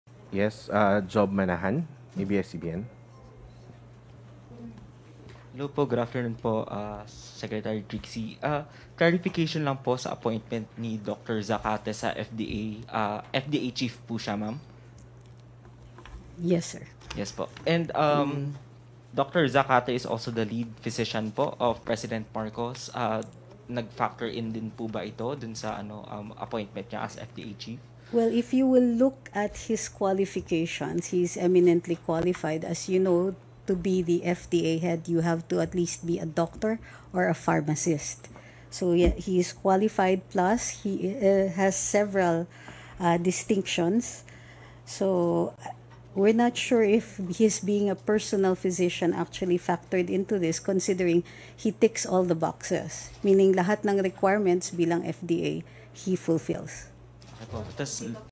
Marcos itinalaga ang personal na doctor bilang FDA chief
Itinalaga ni president Ferdinand "Bongbong" Marcos Jr. si Dr. Samuel Zacate bilang bagong hepe ng Food and Drug Administration. Si Zacate ang pangunahing doktor ni Marcos Jr.
Video by RTVM